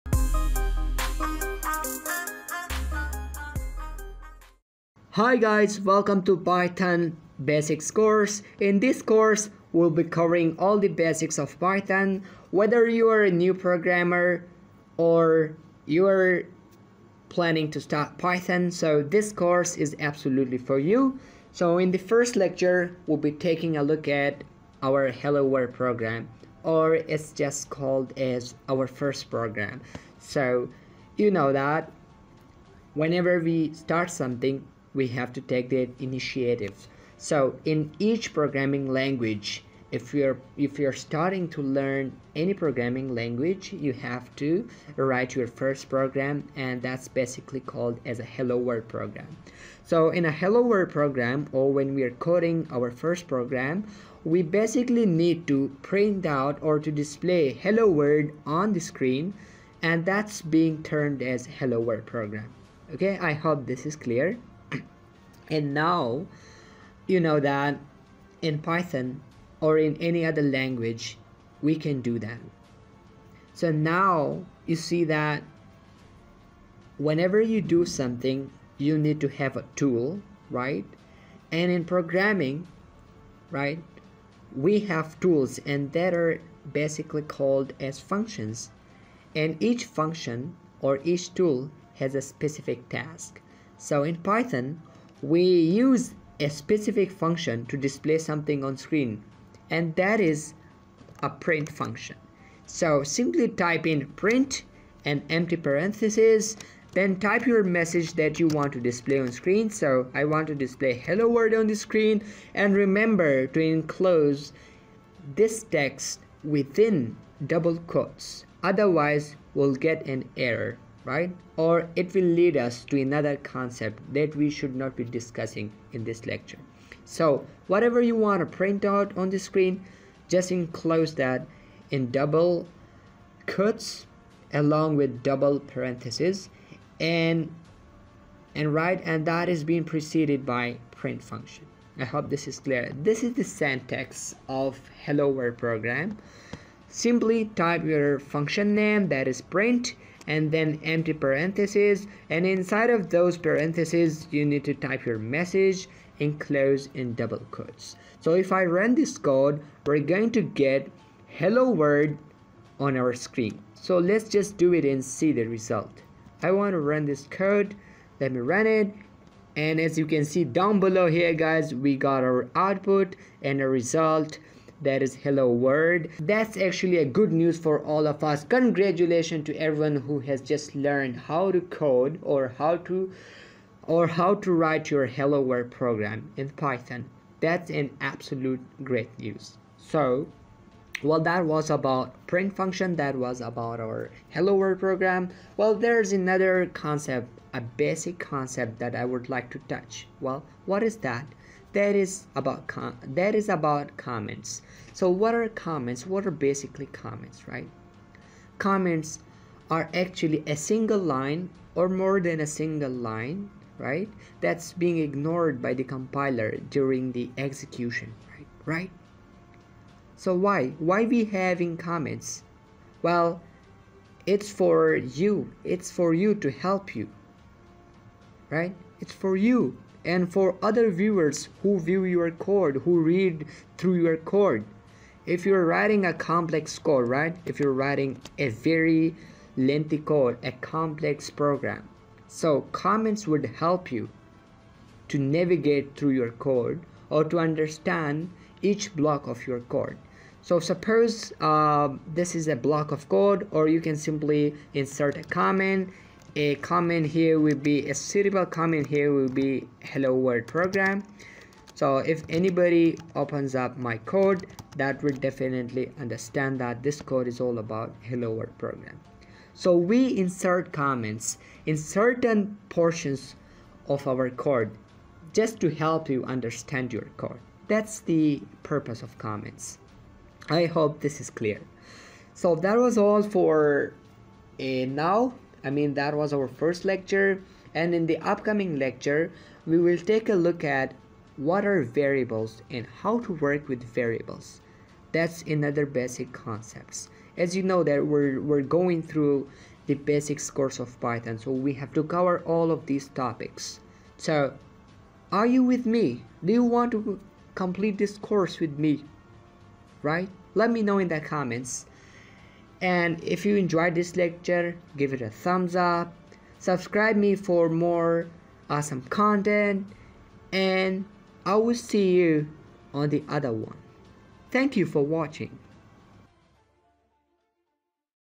Hello World Program in Python !
Welcome to the first video on Python Basics Course for absolute beginners!

We are going to discuss our first program and something about comments too.